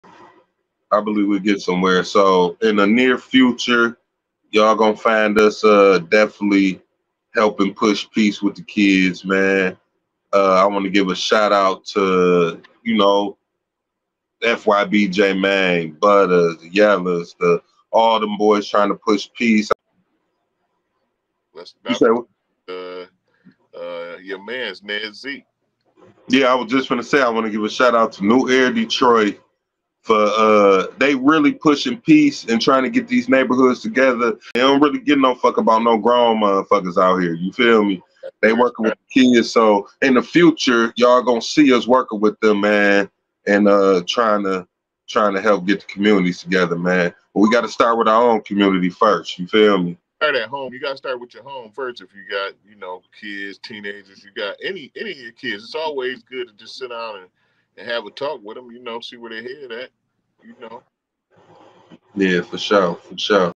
Promoting Peace: Building Stronger Communities with Kids | Ned Z
Join me as we highlight the efforts of FYB, J-Mang, Butta, and the Yellas in pushing peace. Let's give a shout out to New Air Detroit, collaborating with Kenya to bring neighborhoods together. In this video, we discuss the importance of starting at home and having conversations with your kids to create a harmonious community.